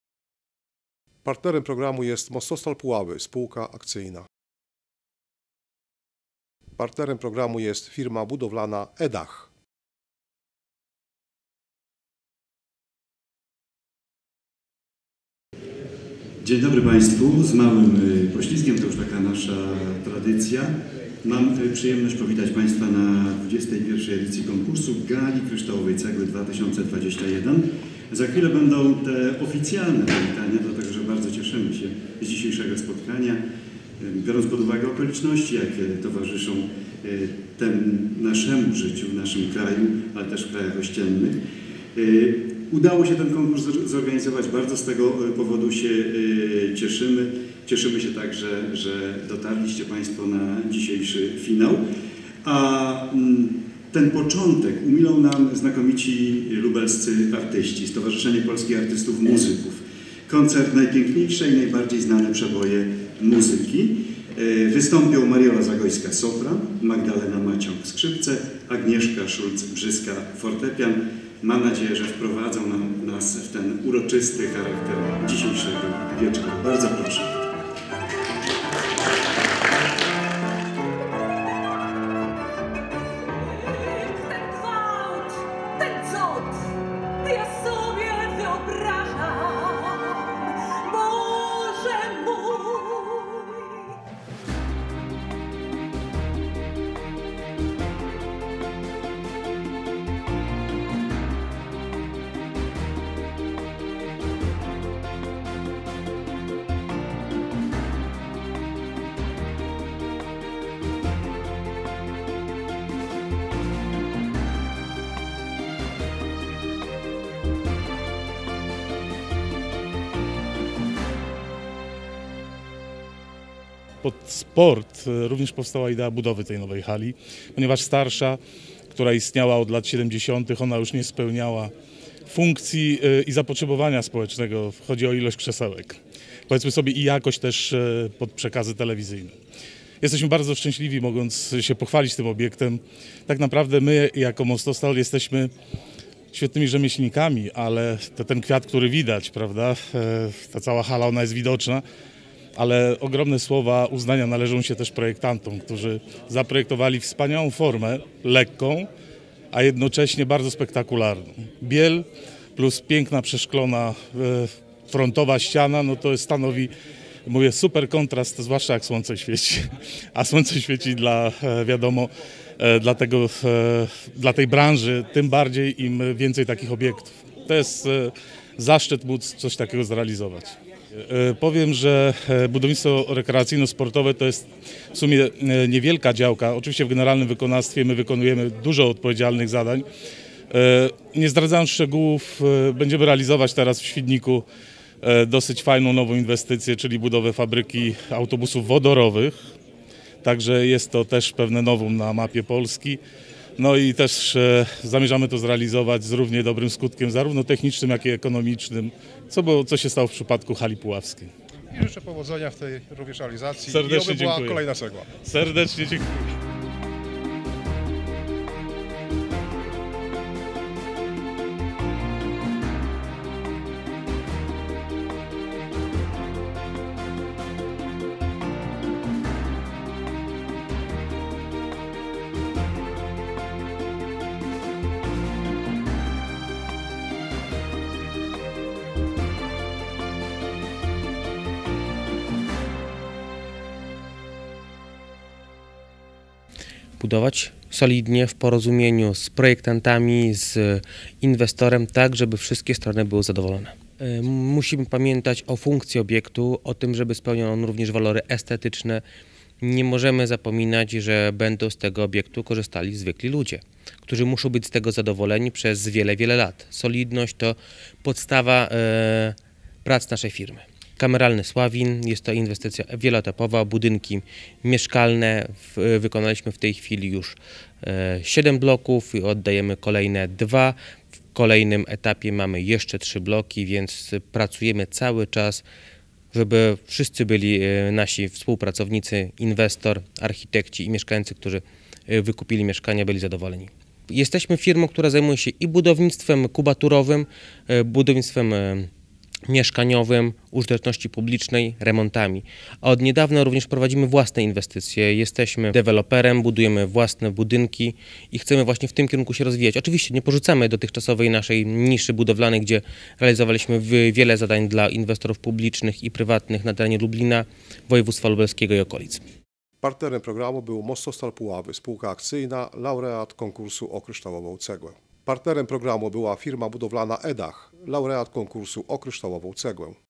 „Kryształowa Cegła” 2022
Z przyjemnością informujemy, że Mostostal Puławy S.A. odebrał Kryształową Cegłę za inwestycję Hala widowiskowo – sportowa w Puławach, która zajęła I miejsce w prestiżowym konkursie „O Kryształową Cegłę” w kategorii „Obiekty Sportu i Rekreacji”. 24 czerwca 2022 r. w Centrum Kongresowym UP w Lublinie podczas uroczystej gali nagrodę odebrał wiceprezes – Jerzy Świeca.